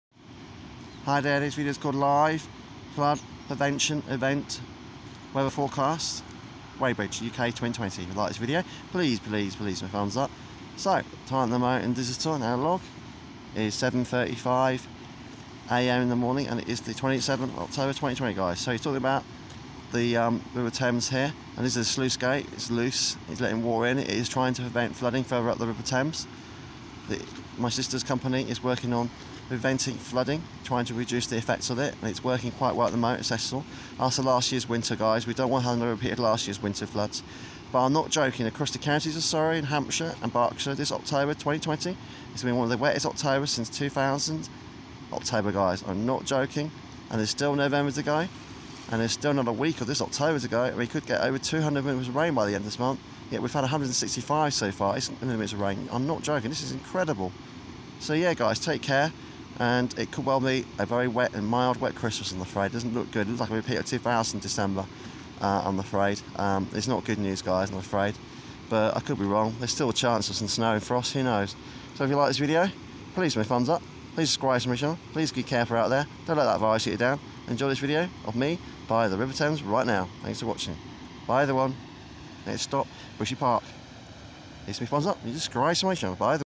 LIVE FLOOD PEVENTION EVENT WEATHER FORECAST UK 2020.
TALK ABOUT THE SLUICE GATES FROM PEVENTING THE RIVER THAMES FROM FLOODING ACROSS THE COUNTY OF SURREY AND THE UK FOR OCTOBER 2020 NO JOKE.